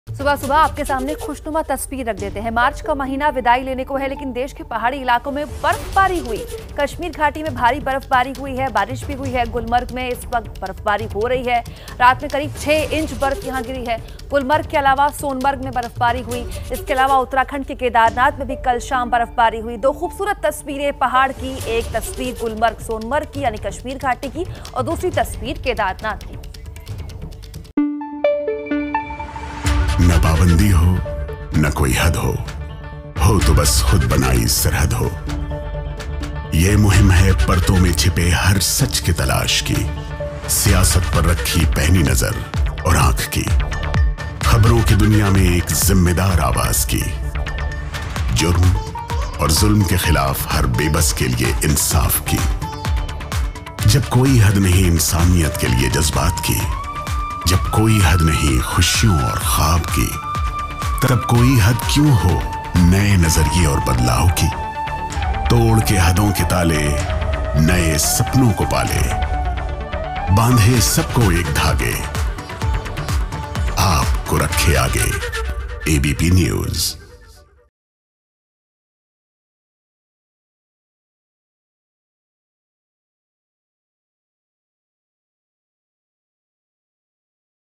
kashmir घाटी और Kedarnath में बर्फबारी
kashmir घाटी और Kedarnath में बर्फबारी. गुलमर्ग और सोनमर्ग में भी बर्फबारी हुई.